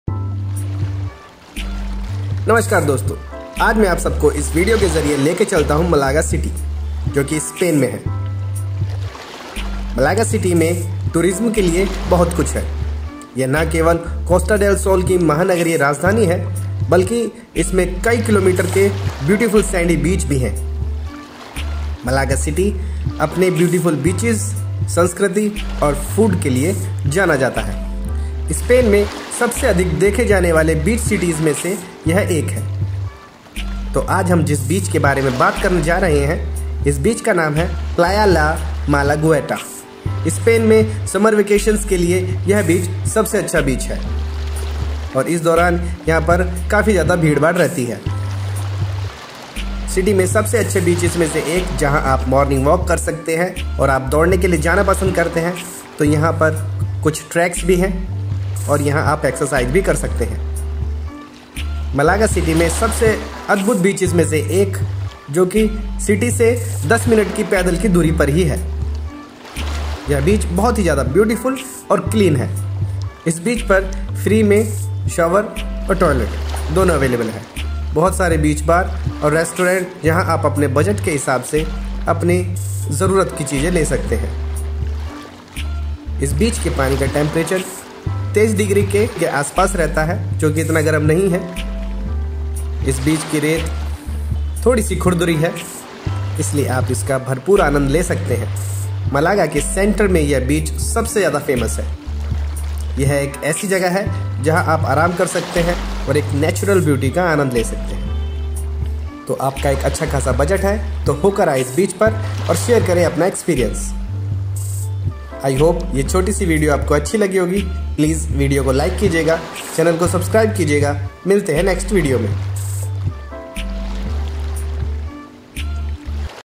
2023 🇪🇸 Malaga~Playa la Malagueta summer beach⛱️ walk 🩴 | malaga city
2023 🇪🇸 Malaga~Playa la Malagueta summer beach⛱️  walk 🩴| Malaga beach walk | malaga city

मलागा शहर में पर्यटकों को देने के लिए बहुत कुछ है, न केवल यह कोस्टा डेल सोल की महानगरीय राजधानी है बल्कि इसमें कई किलोमीटर के सुंदर रेतीले समुद्र तट भी हैं।अपने आश्चर्यजनक समुद्र तटों, संस्कृति और भोजन के लिए जाना जाता है, मलागा अंडालूसिया, स्पेन में सबसे अधिक देखे जाने वाले तटीय शहरों में से एक है। यह स्पेन में गर्मी बिताने के इच्छुक यात्रियों के लिए एक गंतव्य है और गर्म (गर्म) महीनों के दौरान व्यस्त हो सकता है।शहर में सबसे अच्छी समुद्र तटीय सैर में से एक, विशेष रूप से सुबह-सुबह, यदि आप दौड़ने के लिए जाना पसंद करते हैं, तो वहां कुछ स्थान भी हैं जहां आप व्यायाम कर सकते हैं या बस बैठने के लिए देख सकते हैं।शहर के केंद्र और शहर के पूर्व की ओर के बीच भी एक महान संबंध है। मलागा शहर में अद्भुत समुद्र तट, शहर के केंद्र से 10 मिनट की पैदल दूरी पर। समुद्र तट सुंदर, स्वच्छ है, आप मुफ्त स्नान और शौचालय पा सकते हैं, बहुत सारे समुद्र तट बार और रेस्तरां अच्छी कीमत और मोटे सूरज के लिए पानी 23 डिग्री के बारे में इतना गर्म नहीं है और समुद्र तट ज्यादातर रेत और बहुत छोटे पत्थरों के साथ हैं! इसलिए हम इसका भरपूर और निश्चित आनंद लेते हैं! मलागा के मध्य में प्रसिद्ध समुद्र तट। यह एक ऐसी जगह है जहां आप आराम कर सकते हैं और दृश्यों का आनंद ले सकते हैं। बहुत सारे बीच शावर, वॉकवे रेस्त्रां के साथ एक प्राकृतिक अनछुआ समुद्र तट है! रेत खुरदरी होती है लेकिन तेज नहीं!